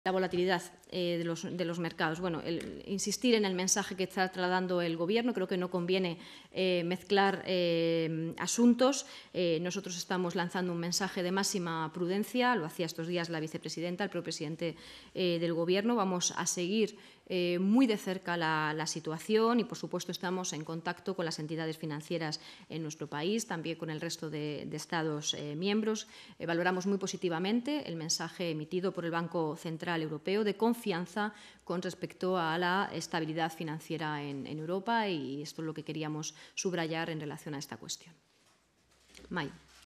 El Gobierno pide "prudencia" y "cautela" ante una posible crisis financiera
La portavoz del Gobierno y ministra de Política Territorial, Isabel Rodríguez, ha lanzado un mensaje de "prudencia" y "cautela" ante un escenario de posible crisis financiera después de la reciente desaparición de Silicon Valley Bank y de que UBS haya comprado Credit Suisse por unos 3.307 millones de euros, lo que ha provocado episodios de tensión en los mercados durante las últimas jornadas.